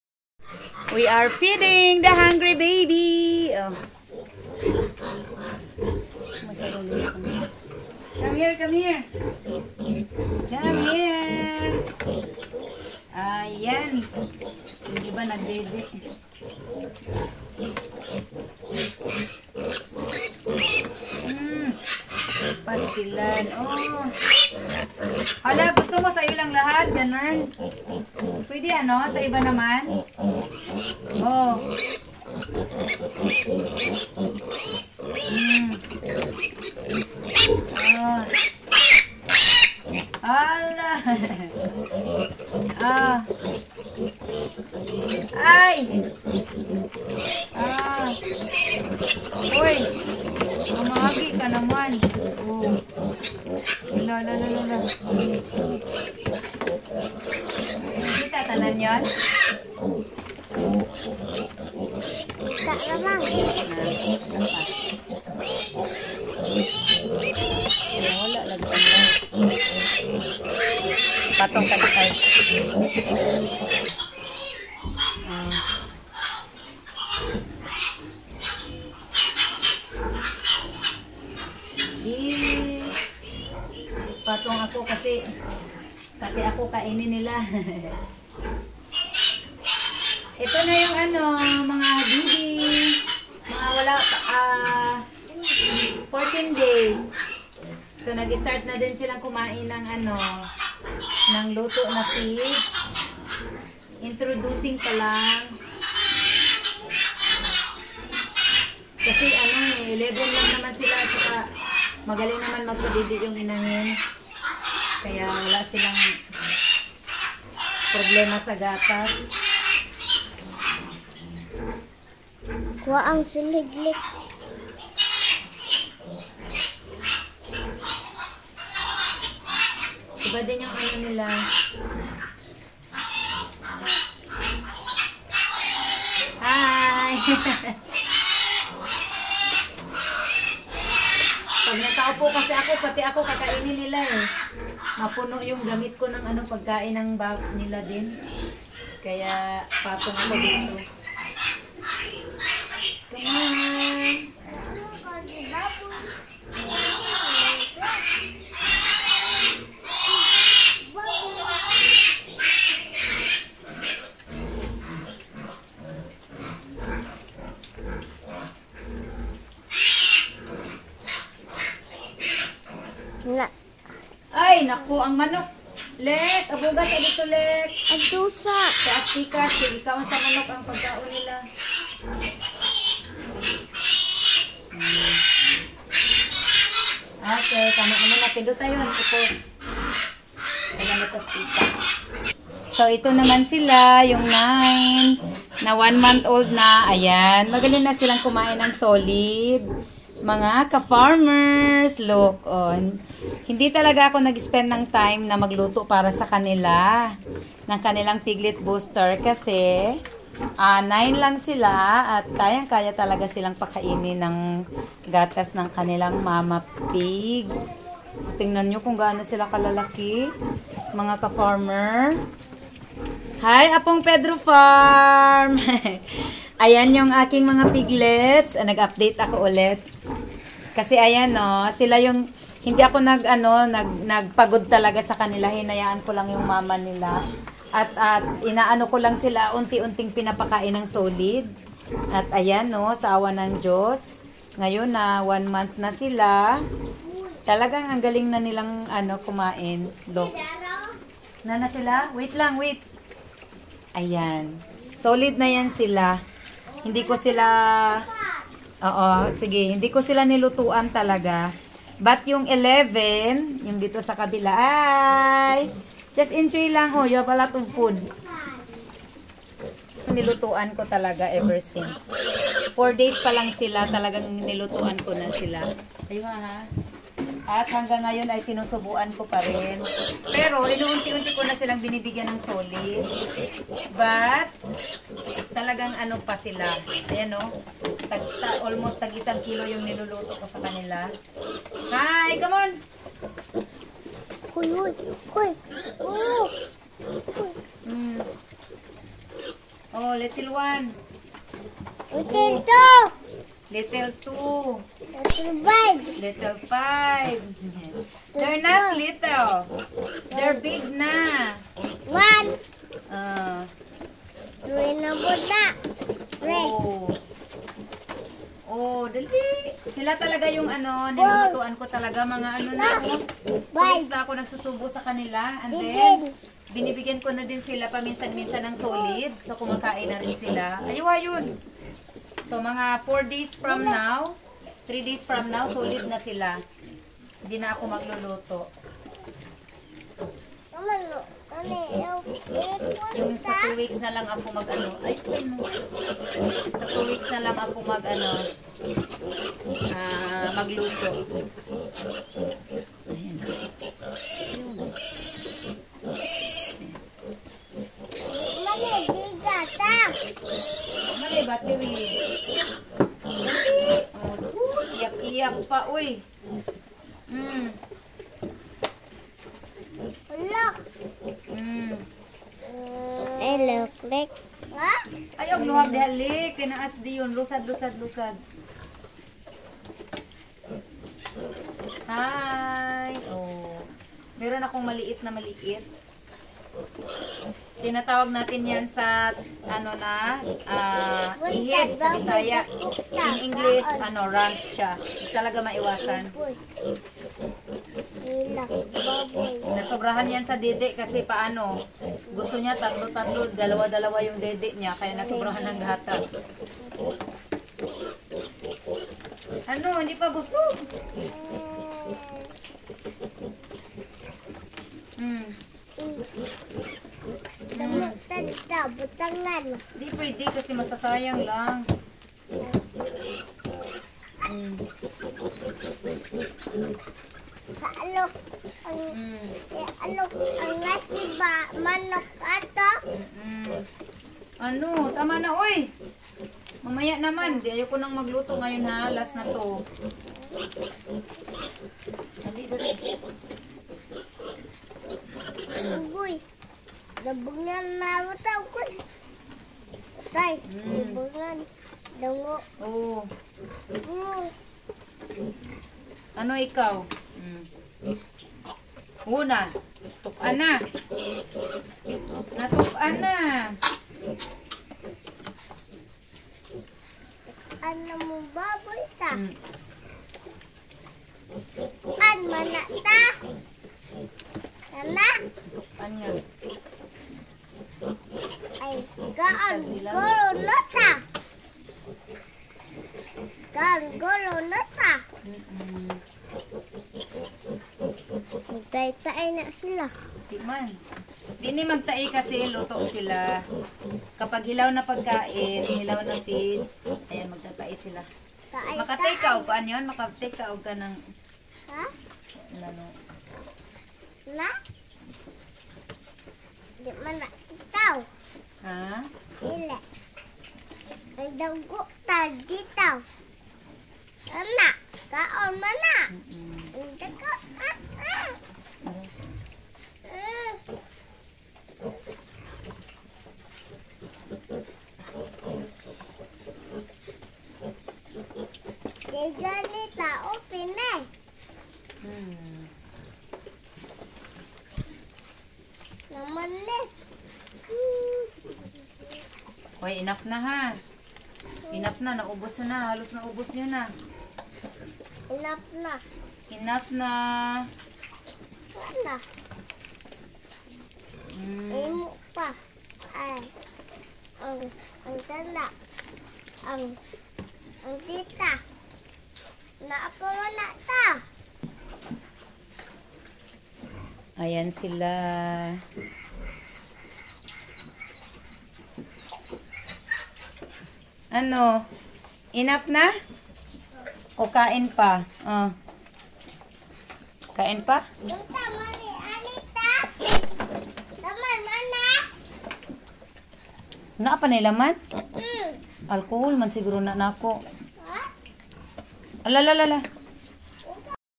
Piglets Update/Ang Bilis Lumaki Ng Aking Mga Biik#philippines
Satisfying pig farmer
its just so nice to see that our piglets grows so fast healthy and strong